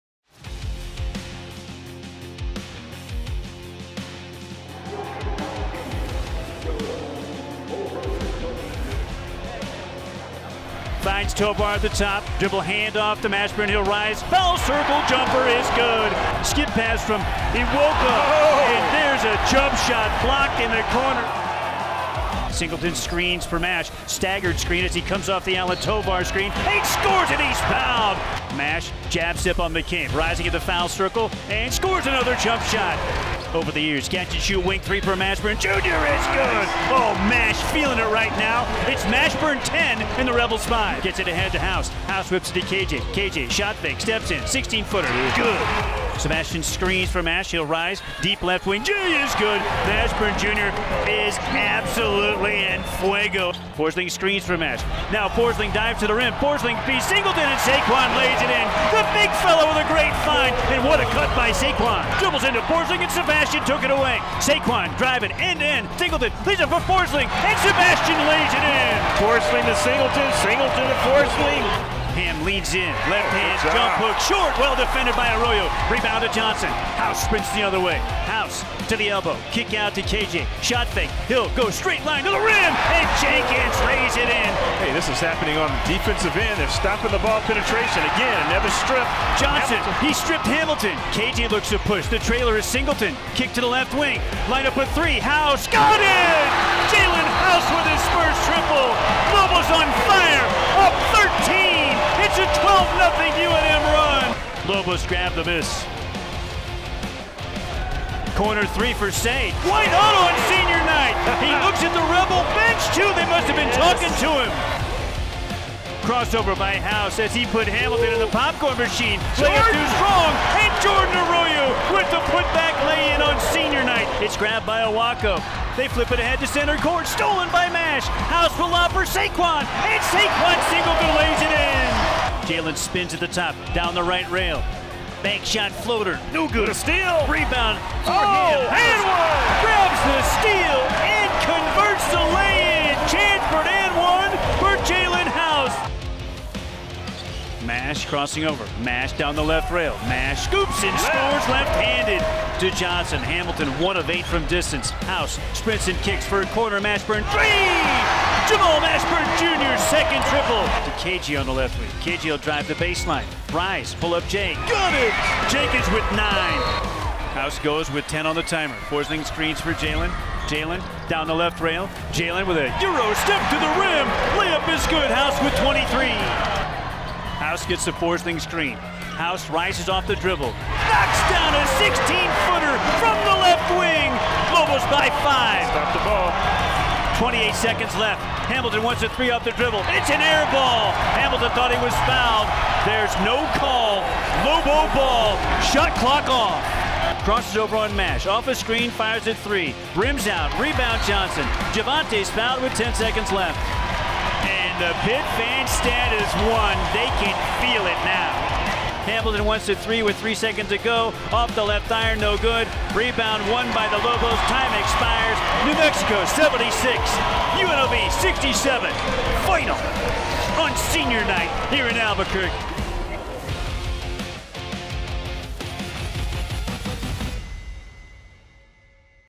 UNM MBB vs. UNLV Highlights (3/5/2022)
Highlights from UNM's 76-67 win over UNLV on senior night!